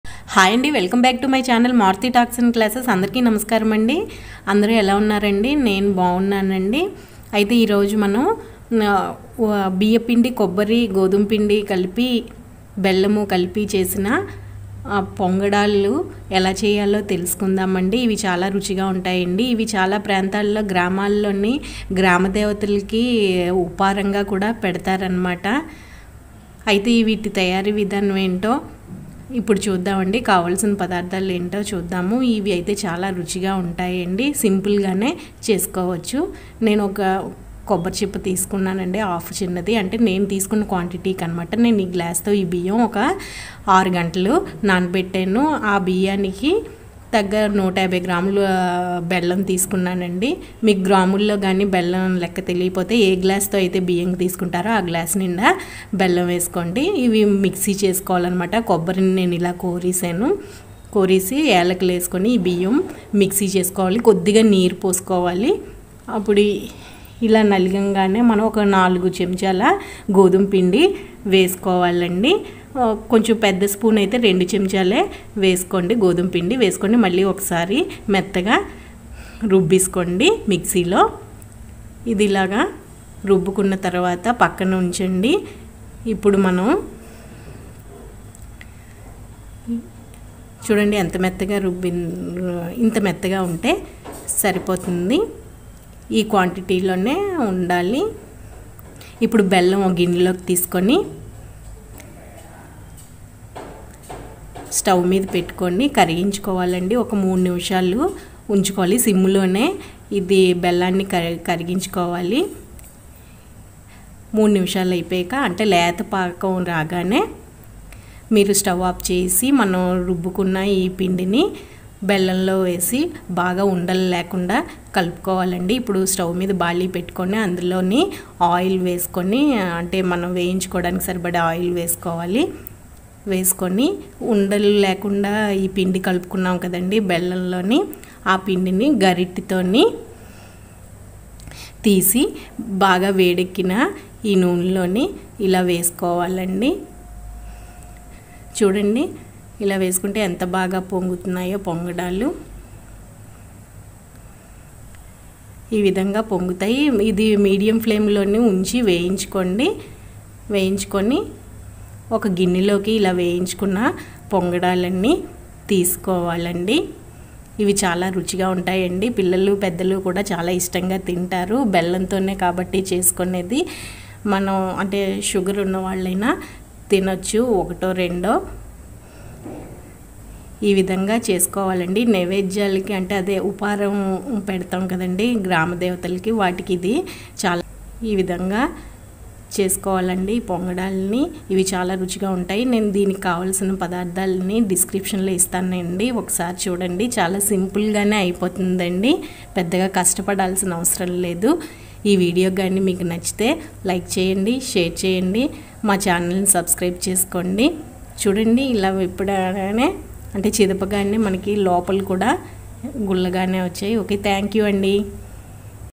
బెల్లం, బియ్యం, కొబ్బరి కలిపి ఈ విదంగా sweet చేసి చూడండి 2023 || bellam పొంగడాలు
belam biyyam pindi kalipi e vidanga sweet cheyandi || నోట్లో పెట్టుకుంటే కరిగి పోయే sweet ||by maruthi talks & classes

kavalasina padardalu

biyyam kobbari, godhuma pindi

bellam, oil